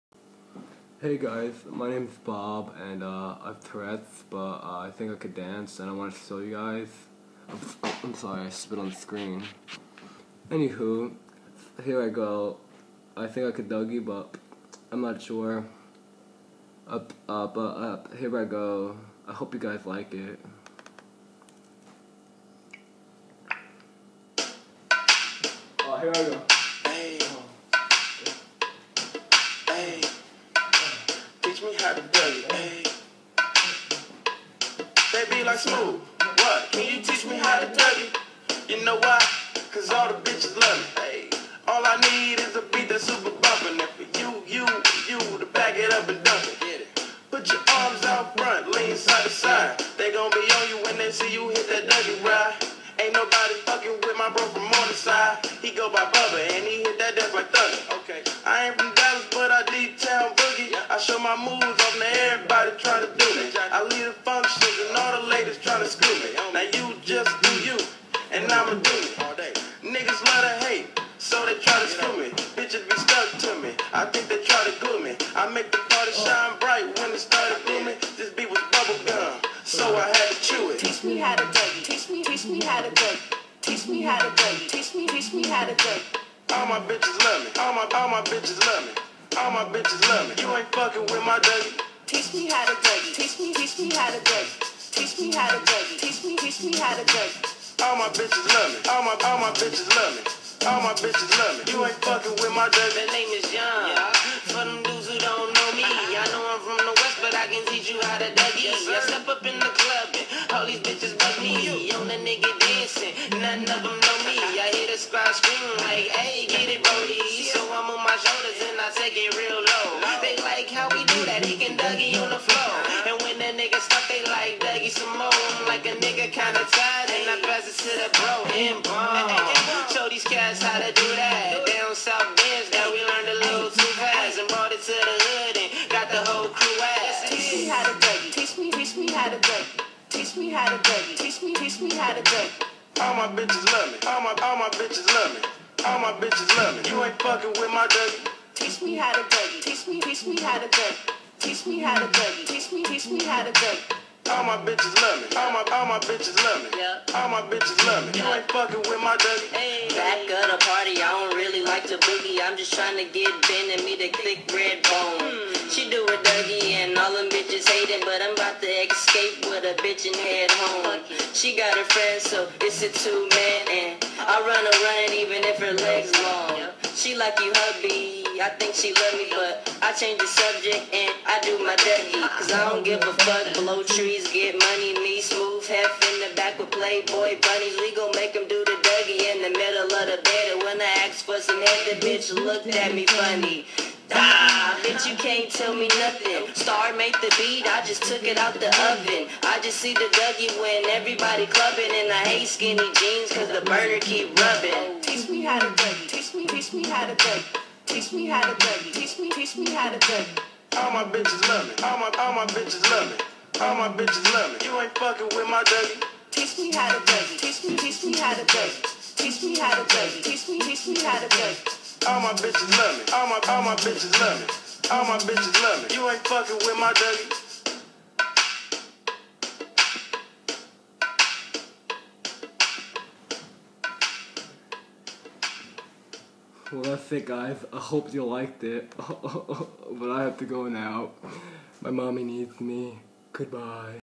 Bob dancing
I made up a character hahaha but trust me I can actually dance I was just bored